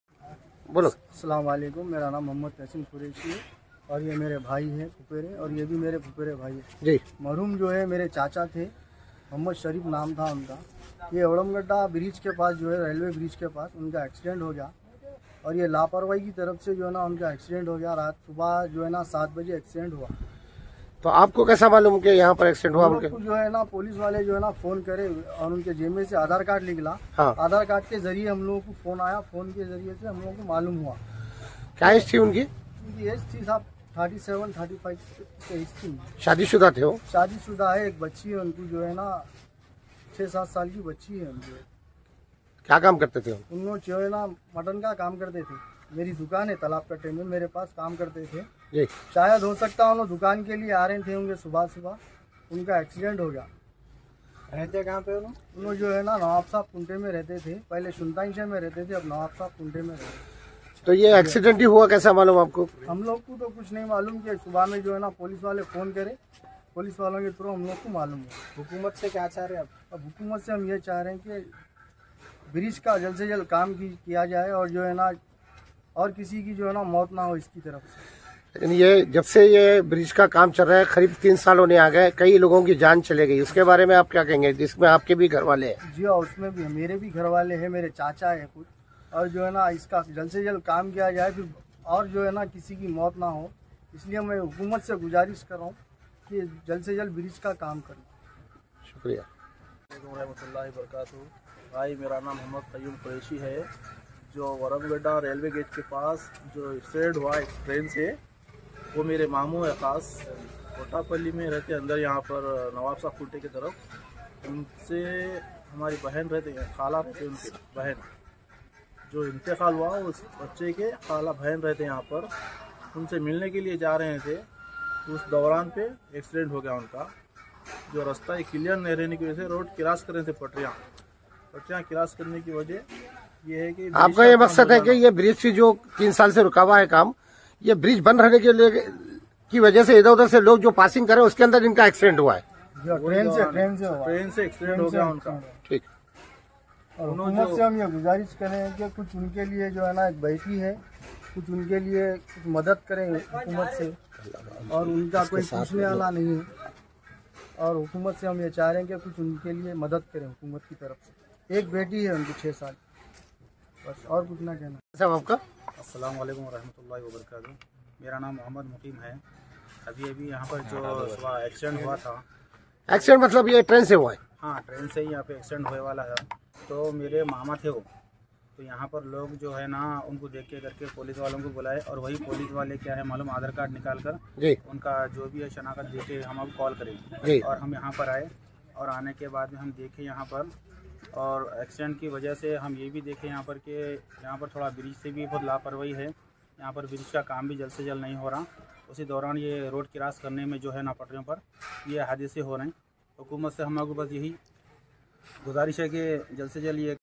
Train Hadese meh Ek Naujawan ka Hua Inteqaal | Crossing ke Waqt Bahoot Ehtiyath kerre | ALLAH | Rail
Train Hadese meh Ek Naujawan ka Hua Inteqaal, Crossing ke Waqt Bahoot Ehtiyath kerre

ALLAH, Hum Saab ki Hifazat Kerre